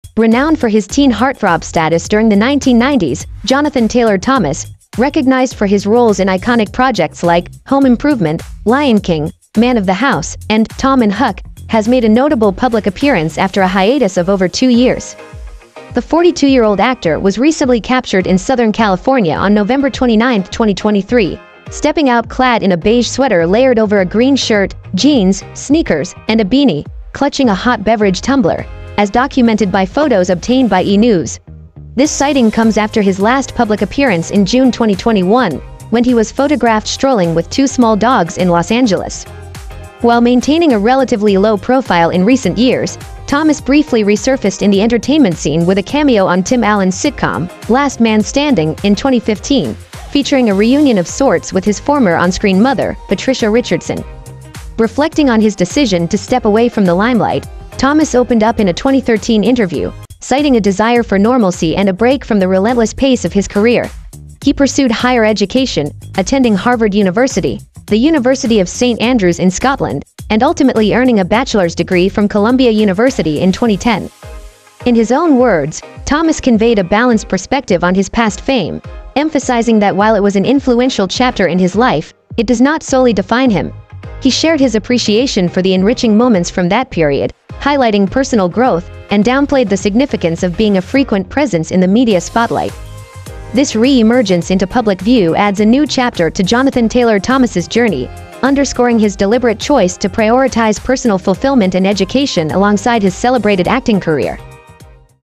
Home Improvement' Alum Jonathan Taylor Thomas Spotted for the First Time in 2 Years
Jonathan Taylor Thomas, 90s Heartthrob and Former Child Star, Resurfaces After Two-Year Absence

Renowned for his teen heartthrob status during the 1990s, Jonathan Taylor Thomas, recognized for his roles in iconic projects like "Home Improvement," "Lion King," "Man of the House," and "Tom and Huck," has made a notable public appearance after a hiatus of over two years.

The 42-year-old actor was recently captured in Southern California on November 29, 2023, stepping out clad in a beige sweater layered over a green shirt, jeans, sneakers, and a beanie, clutching a hot beverage tumbler, as documented by photos obtained by E! News. This sighting comes after his last public appearance in June 2021, when he was photographed strolling with two small dogs in Los Angeles.

While maintaining a relatively low profile in recent years, Thomas briefly resurfaced in the entertainment scene with a cameo on Tim Allen's sitcom "Last Man Standing" in 2015, featuring a reunion of sorts with his former on-screen mother, Patricia Richardson.

Reflecting on his decision to step away from the limelight, Thomas opened up in a 2013 interview, citing a desire for normalcy and a break from the relentless pace of his career. He pursued higher education, attending Harvard University, the University of St Andrews in Scotland, and ultimately earning a bachelor's degree from Columbia University in 2010.

In his own words, Thomas conveyed a balanced perspective on his past fame, emphasizing that while it was an influential chapter in his life, it does not solely define him. He shared his appreciation for the enriching moments from that period, highlighting personal growth, and downplayed the significance of being a frequent presence in the media spotlight.

This re-emergence into public view adds a new chapter to Jonathan Taylor Thomas's journey, underscoring his deliberate choice to prioritize personal fulfillment and education alongside his celebrated acting career.